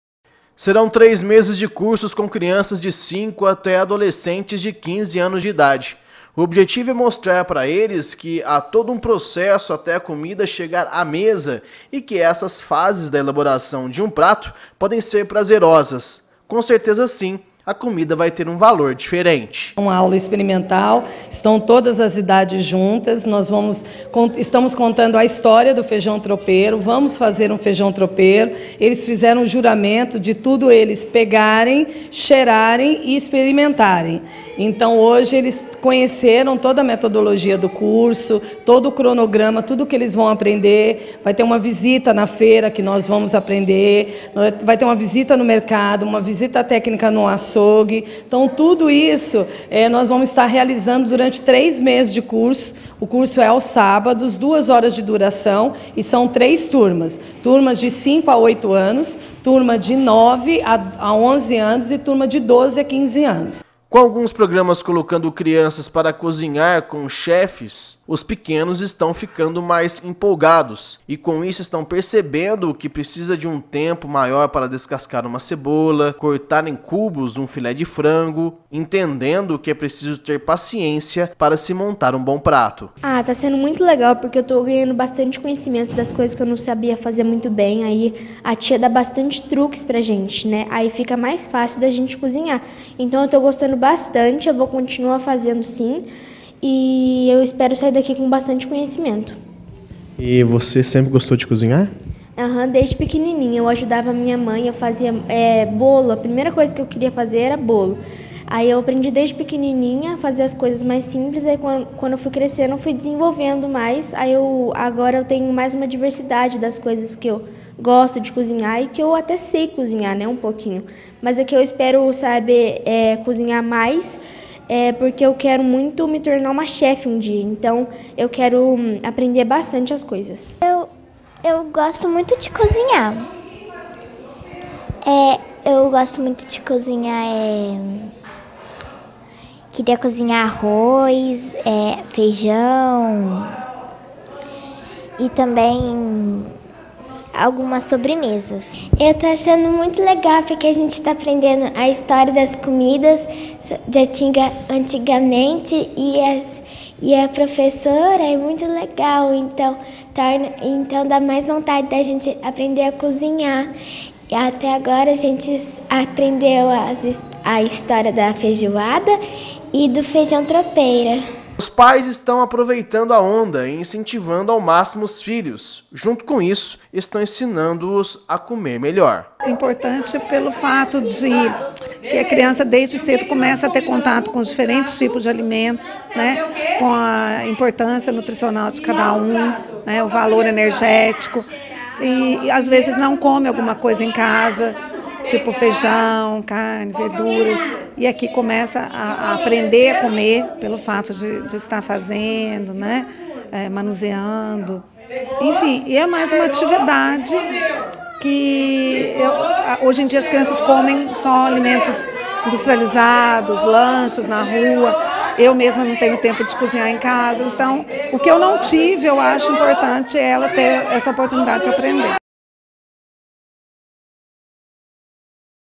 CURSO DE CULINÁRIA INFANTIL
Curso de culinária infantil que aproximar as crianças com as formas de se fazer comida e afastá-las dos fast foods da vida.